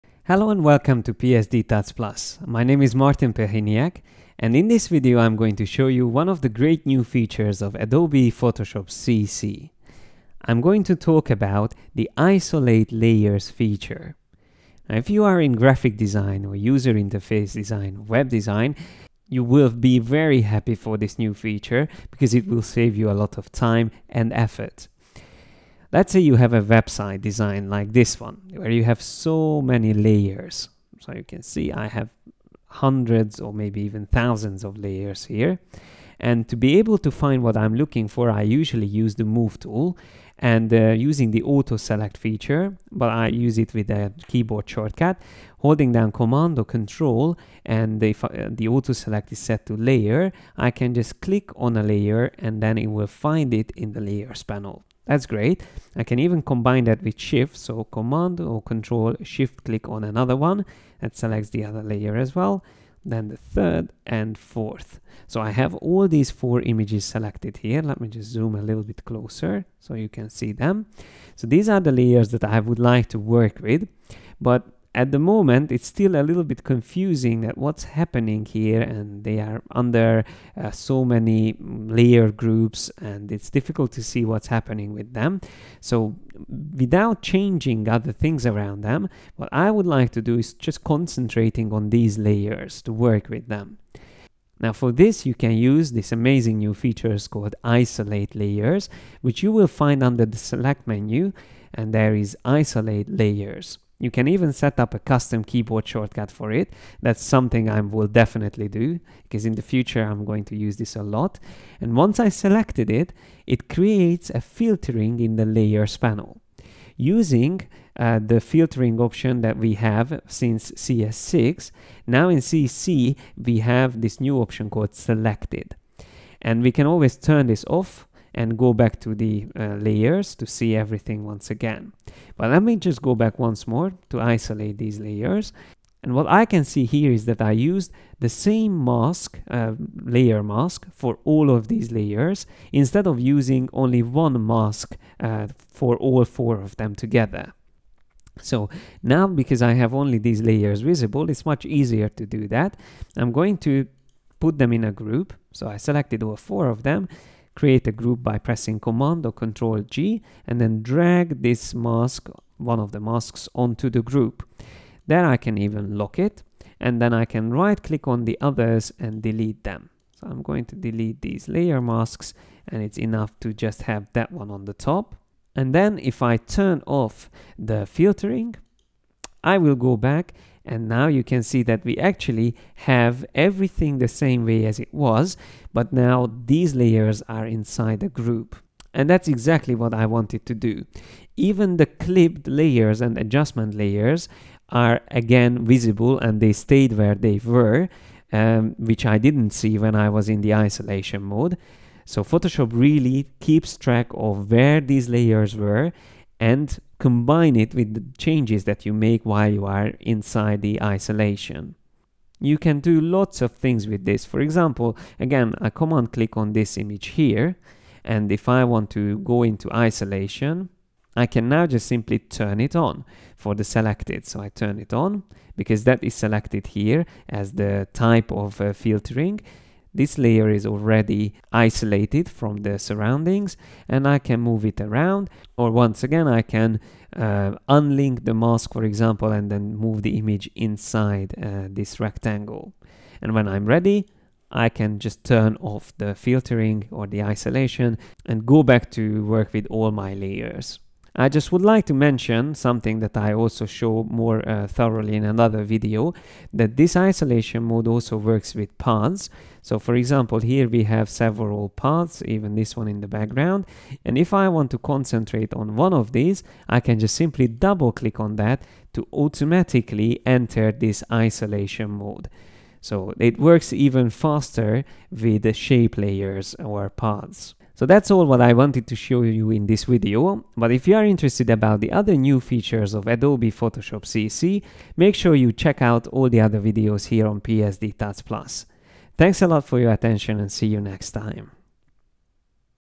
How to Isolate Layers in Photoshop CC
A fantastic new way to simplify your work is to Isolate layers in a complicated layer structure. This feature allows to concentrate on a specific layer or set of layers and don't get confused by all the other layers.